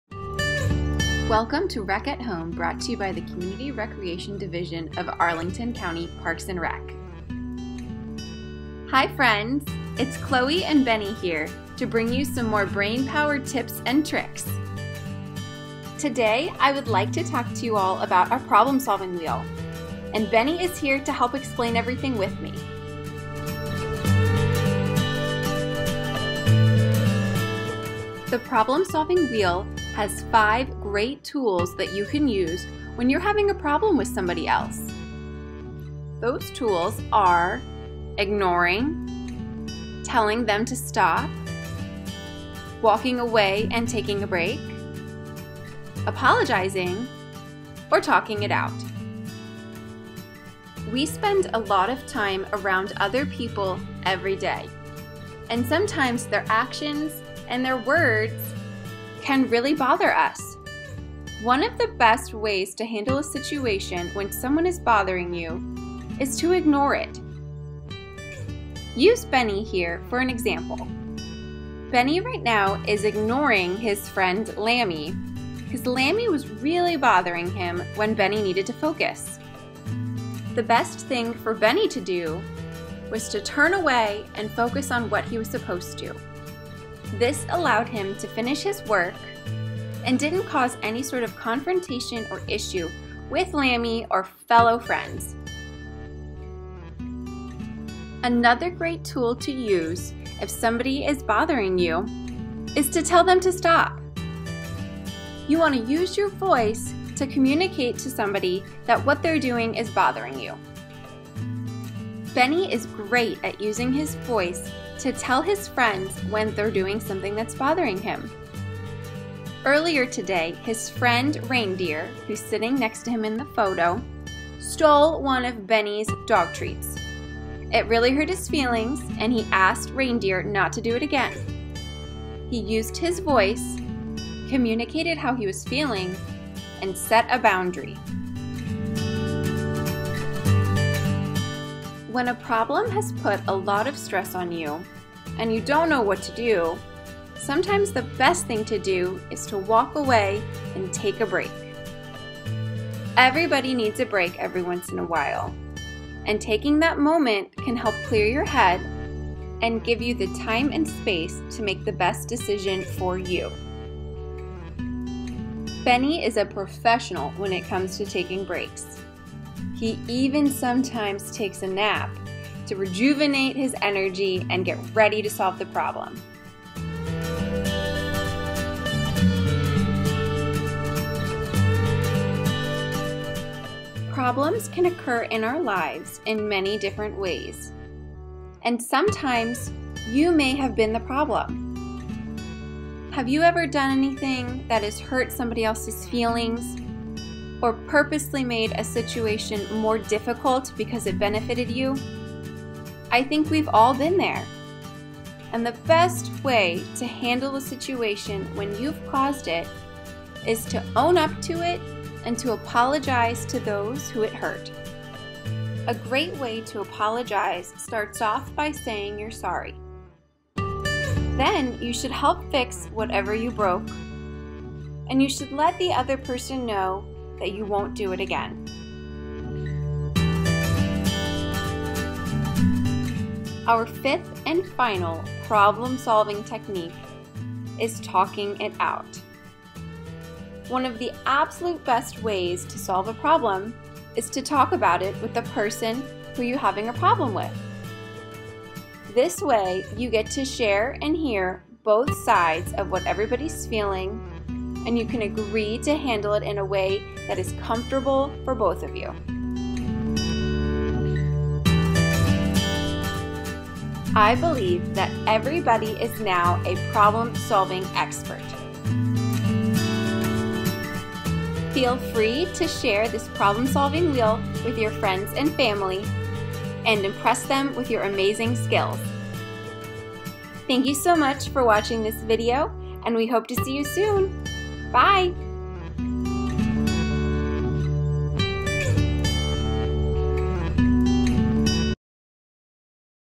Problem Solving Wheel!
Help children make positive decisions the next time they're having a problem with a sibling, friend or neighbor. Ms. Chloe is teaching how to use a social-emotional tool called the Problem Solving Wheels.